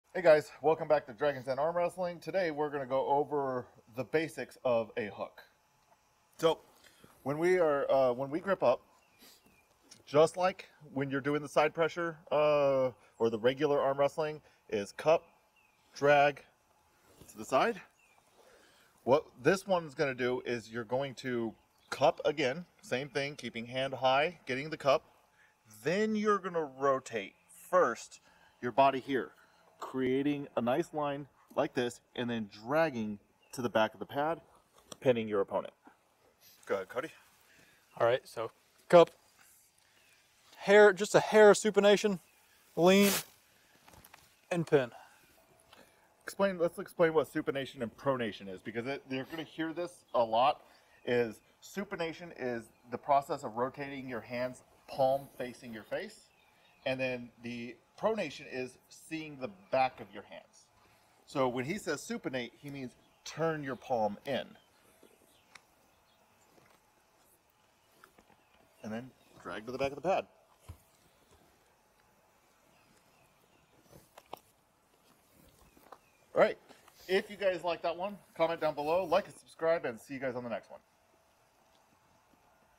Armwrestling Masterclass | The Basic Hook | Dragons Den University
Dragons Den University builds lectures on how to do the variety of techniques for armwrestling, this training is going to teach the Hook in the most basic form of the technique.